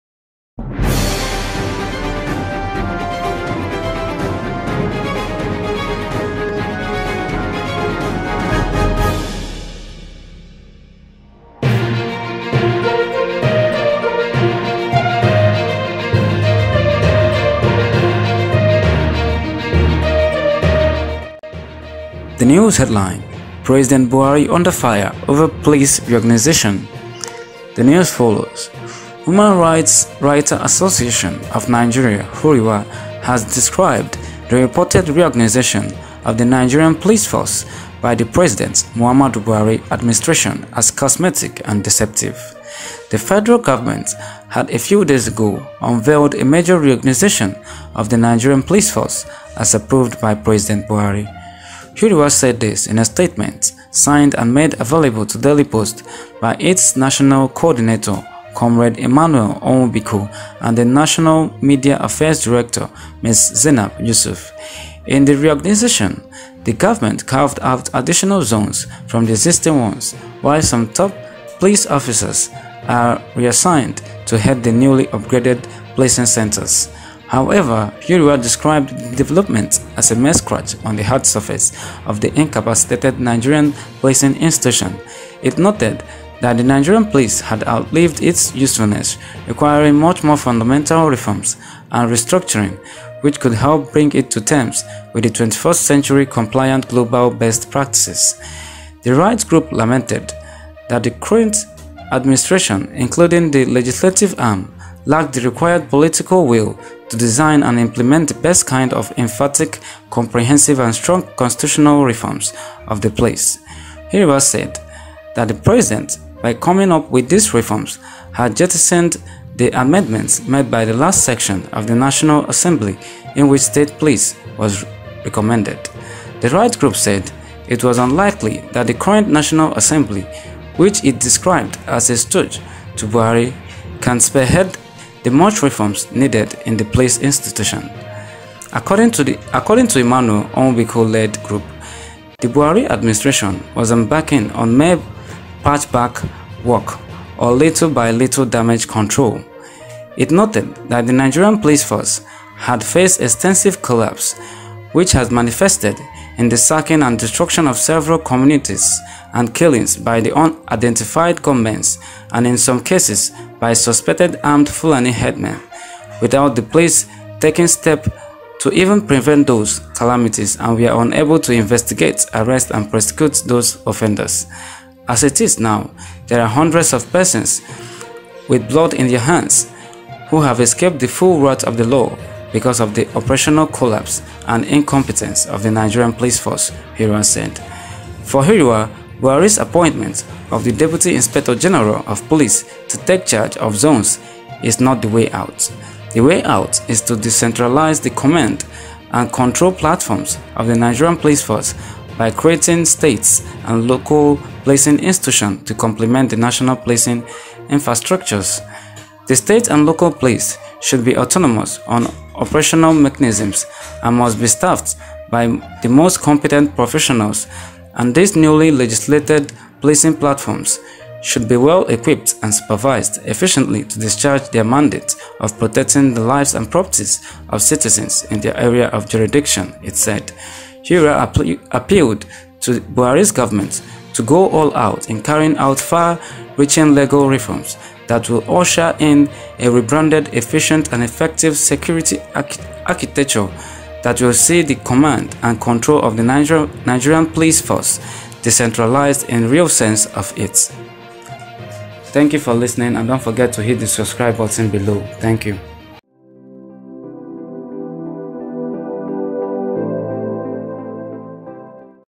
President Buhari under fire over police reorganisation
Human Rights Writers Association of Nigeria, HURIWA, has described the reported reorganisation of the Nigerian Police Force by the President Muhammadu Buhari administration as cosmetic and deceptive.

The Federal Government had a few days ago unveiled a major reorganisation of the Nigerian Police Force as approved by President Buhari.

HURIWA said this in a statement signed and made available to DAILY POST by its National Coordinator, Comrade Emmanuel Onwubiko and the National Media Affairs Director, Miss Zainab Yusuf.

In the reorganisation, the government carved out additional zones from the existing ones, while some top police officers are re-assigned to head the newly upgraded policing centres.

However, HURIWA described the development as a mere scratch on the hard surface of the incapacitated Nigerian policing institution.

It noted that the Nigerian police had outlived its usefulness, requiring much more fundamental reforms and restructuring which could help bring it to terms with the twenty-first century compliant global best practices.

The Rights group lamented that the current administration, including the legislative arm, lacked the required political will to design and implement the best kind of emphatic, comprehensive and strong constitutional reforms of the police.

HURIWA said that President Buhari, by coming up with these reforms, had jettisoned the amendments made by the last session of the National Assembly, in which state Police was recommended.

The Rights group said it was unlikely that the current National Assembly, which it described as a stooge to Buhari, can spearhead the much reforms needed in the police institution.

According to the Emmanuel Onwubiko-led group, the Buhari administration was embarking on mere ‘patch patch’ work or little by little damage control.

It noted that the Nigerian Police Force had faced extensive collapse which has manifested in the sacking and destruction of several communities and killings by unidentified gunmen and in some cases by suspected armed Fulani herdsmen without the police taking steps to even prevent those calamities and were unable to investigate, arrests and prosecute these offenders.

“As it is now, there are hundreds of persons with blood in their hands who have escaped the full wrath of the law because of the operational collapse and incompetence of the Nigerian Police Force,” HURIWA said.

For HURIWA, Buhari’s appointment of the Deputy Inspector General of Police to take charge of zones is not the way out.

“The way out is to decentralise the command and control platforms of the Nigerian Police Force by creating State and local policing institutions to compliment the National policing infrastructure.

“The State and local police should be autonomous on operational mechanisms and must be staffed by the most competent professionals and these newly legislated policing platforms should be well equipped and supervised efficiently to discharge their mandate of protecting the lives and property of citizens in their areas of jurisdiction,” it said.

HURIWA appealed to the Buhari government to go all out in carrying out far reaching legal reforms that will usher in a rebranded, efficient and effective security architecture that will see the command and control of the Nigerian Police Force decentralised in the real sense of it.